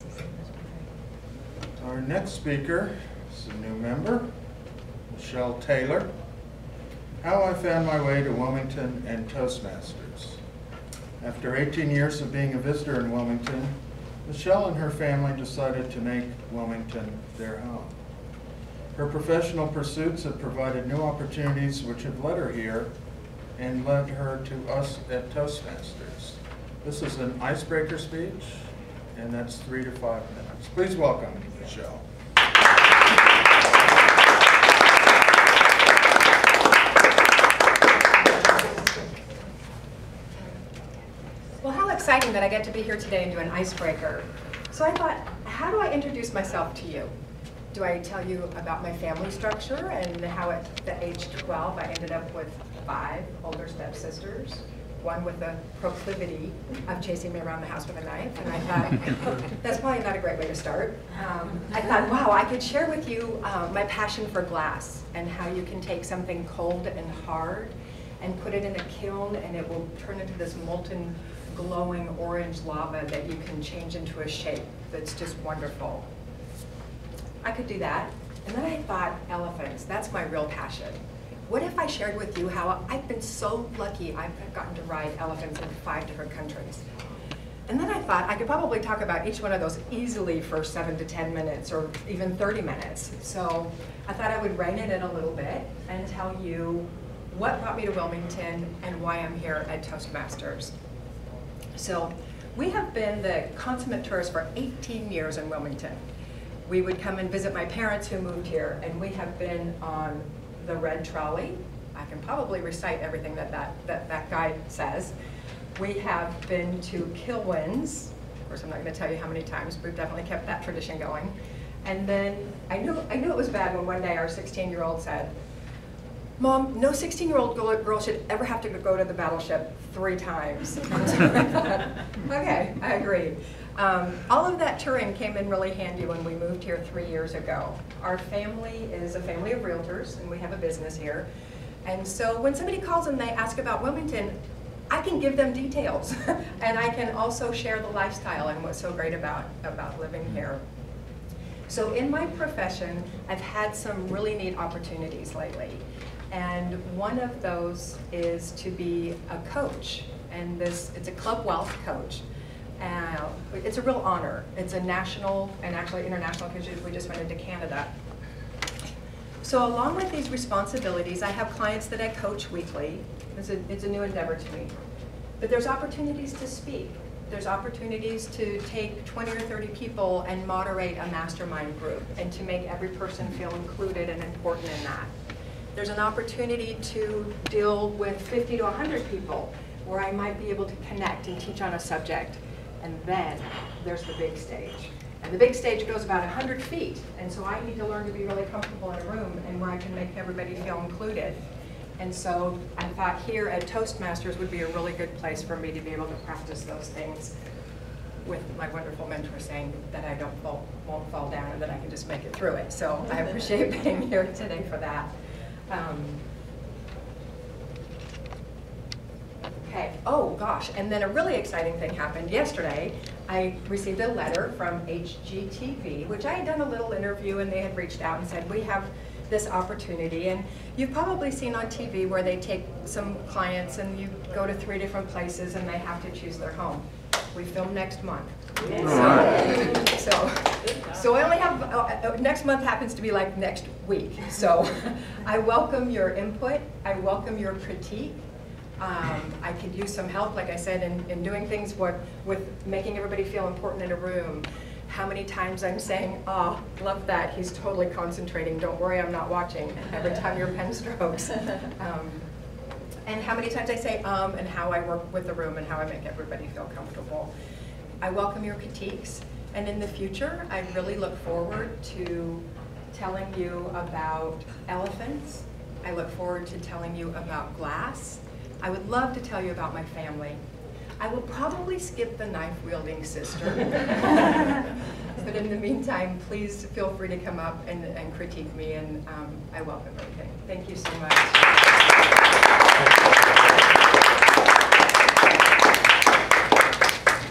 Michelle Taylor, Icebreaker, 6-26-18
Michelle Taylor gave her Icebreaker speech to her Wilmington Toastmasters club on June 26, 2018. Michelle’s Icebreaker is titled, “How I found My Way to Wilmington, NC and Toastmasters.” The Icebreaker is the first speech in the Competent Communicator Manual and allows a new member to tell her fellow club members a little about themselves. Welcome to the club Michelle and thank you for this great first speech.